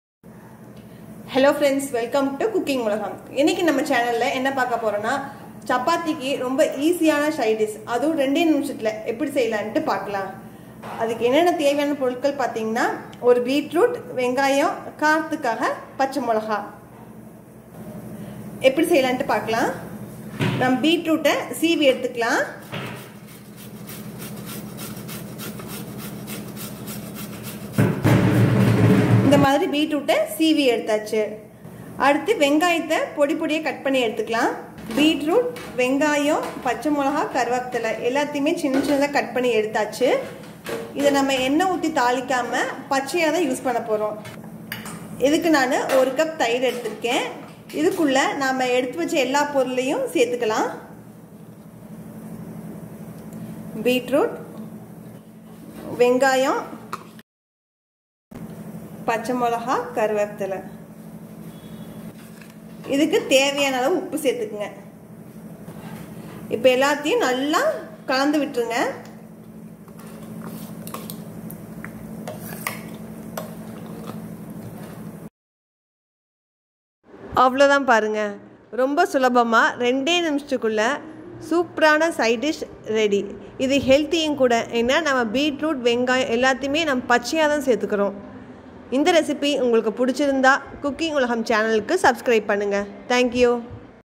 புதுவிதமான ஹெல்த்தி ரெசிபி for Breakfast and Dinner / Best Sidedish for chapathi
side dish for chapathi
Ingredients:
Beetroot  -  1no
Onion  - 1no
Green chilli    - 2nos
Curry leaves